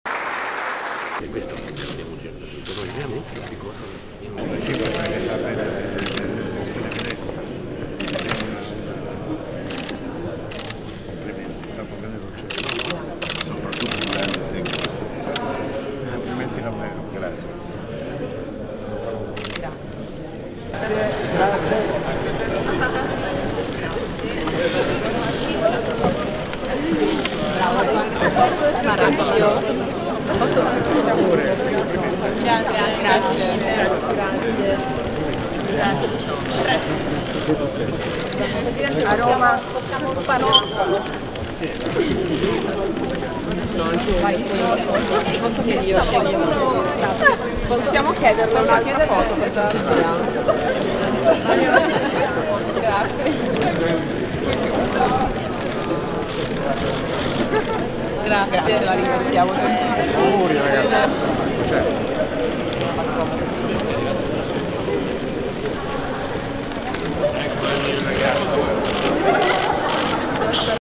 Selfie e strette di mano per Mattarella e gli azzurri della Nazionale di atletica
(Agenzia Vista) Al termine dell'incontro al quirinale, tanti selfie e strette di mano tra Mattarella e gli azzurri della nazionale italiana di atletica leggera.